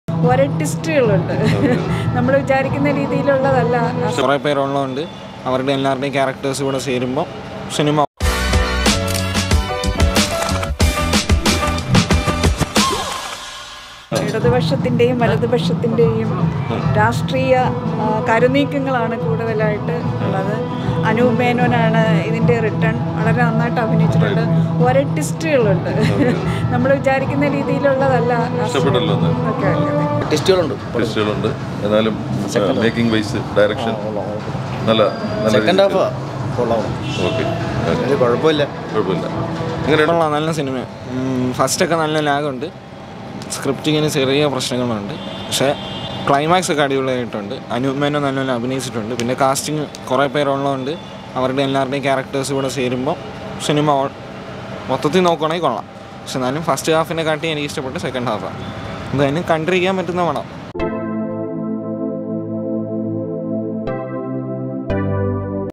VARAAL Movie Review | Theatre Response | Malayalam | Anoop Menon | Kannan Thamarakkulam
VARAAL Malayalam Movie Theatre Response | Varaal Theatre Response | Varaal Public Review

Varaal is a Malayalam drama movie, directed by Kannan Thamarakulam. The cast of Varaal includes Anoop Menon,Prakash Raj.

Set in the year of 2024 and the Kerala election to the assembly is in the offing. The CM Achuthan Nair (Prakash Raj) has been in the office for 10 years. The Delhi high command recommends a newcomer to the post, David John Medayil (Anoop Menon). Will David Medayil survive the onslaught? Will Achuthan Nair emerge as the omnipotent?